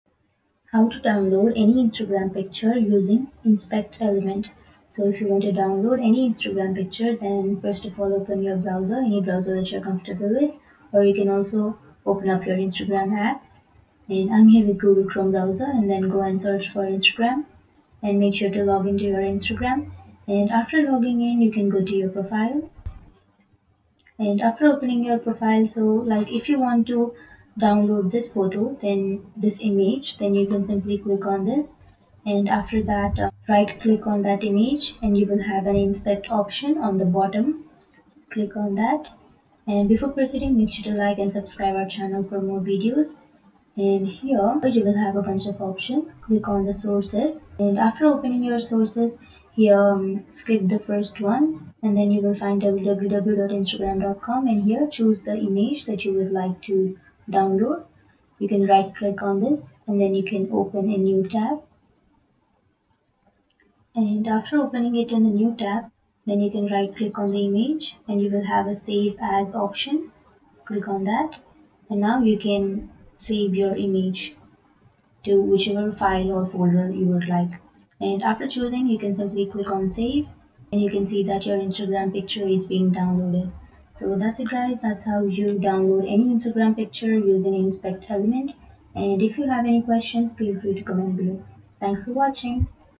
How to Download Any Instagram Picture Using Inspect Element?
Want to download an Instagram picture using the Inspect Element feature? In this tutorial by MadTech, we'll show you how to use the Inspect Element tool to download any Instagram picture easily. While Instagram doesn't provide a direct download option, Inspect Element allows you to access the underlying code of a webpage and extract the image URL. We'll guide you through the step-by-step process, demonstrating how to find the image URL and download it using your web browser. With this handy technique, you can save Instagram pictures for personal use or reference. Watch this tutorial to unlock the Inspect Element method for downloading Instagram pictures with MadTech!